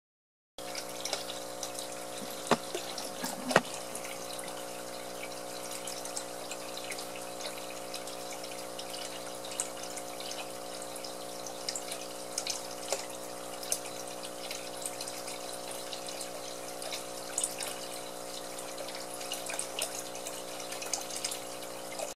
Ants eating a Slug Whole
Small black ants are eating a delicious slug.